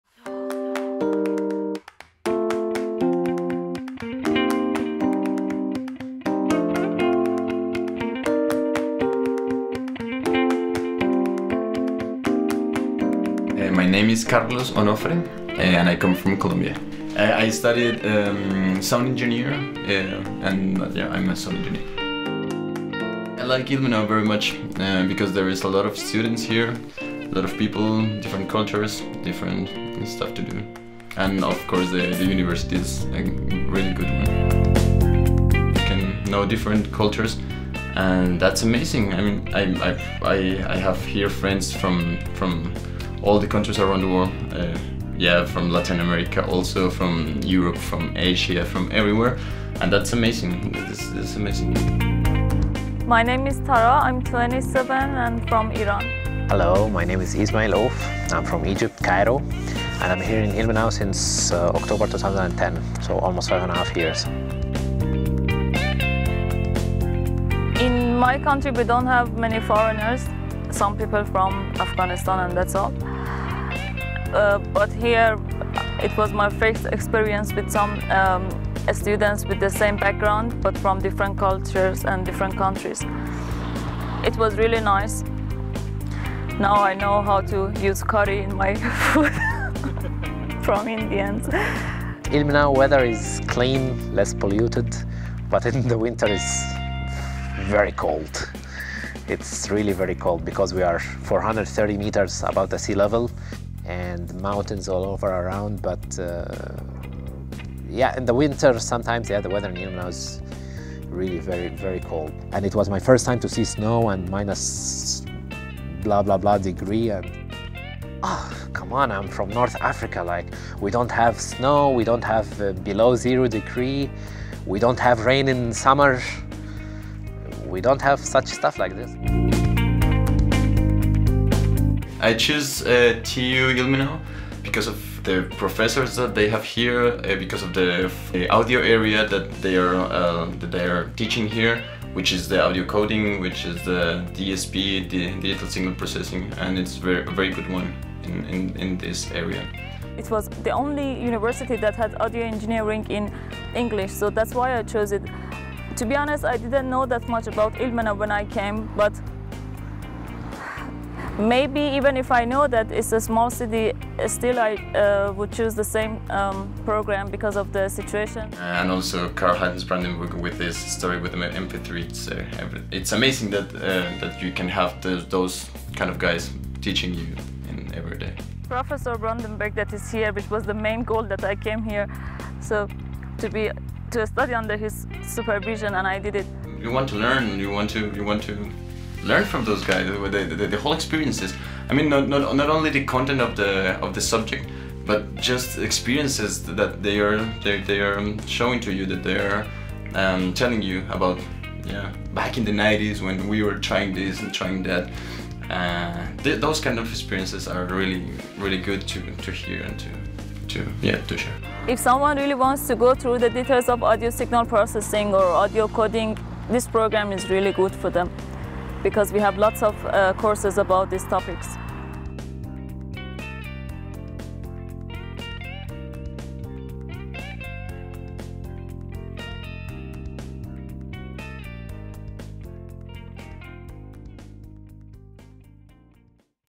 MT: Studying Media Technology - Student Life - TU Ilmenau
English:
The international Master's program Media Technology at the Ilmenau University of Technology attracts students from around the world. In the video some students report from their experience in studying and living in Ilmenau. The video was produced as part of a multimedia project.

German:
Der internationale Master Studiengang Medientechnologie an der Technischen Universität Ilmenau zieht Studierende aus aller Welt an. Im Video berichten einige Studierende von ihren Erfahrungen im Studium und dem Leben in Ilmenau. Das Video entstand im Rahmen eines Multimediaprojekts.